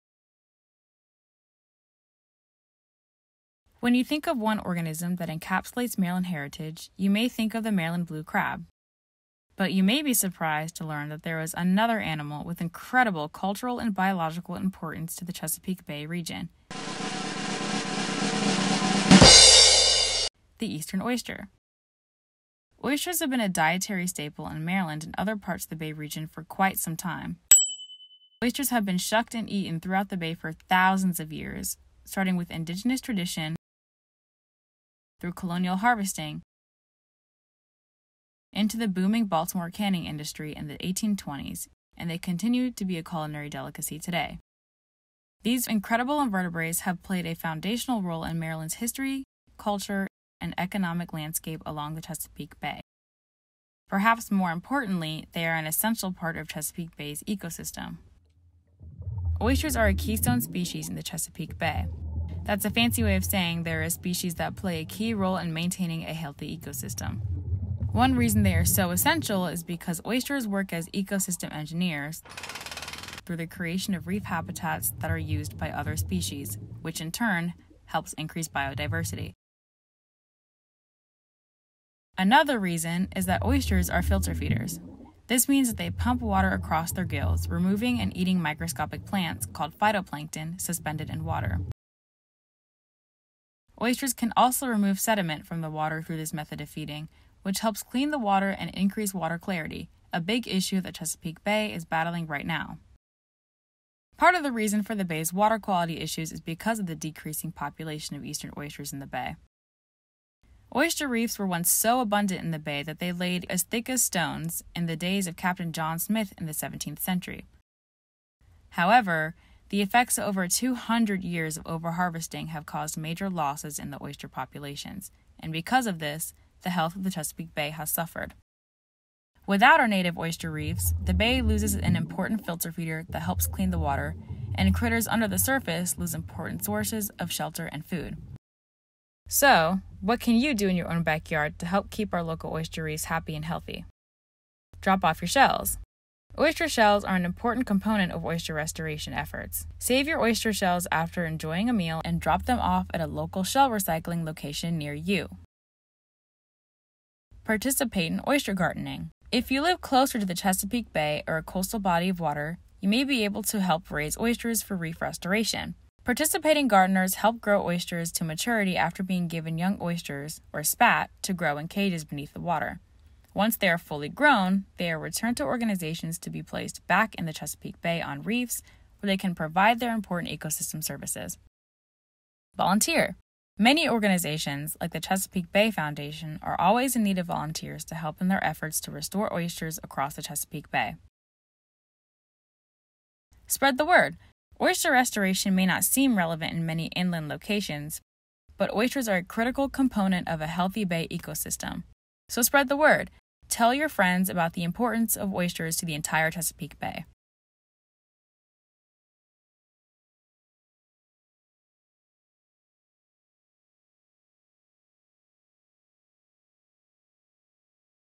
SERC Interns Explain: People, Oysters, and the Chesapeake Bay
When you think of one organism that encapsulates Maryland heritage, you may think of the Maryland blue crab. But you may be surprised to find that there is another animal with incredible cultural and biological importance to the Chesapeake Bay region: the eastern oyster.

Join Vanessa Holloway-Chambers, SERC summer intern 2022, as she describes the history of the Chesapeake Bay oysters and humans, and what you can do to ensure their bright future.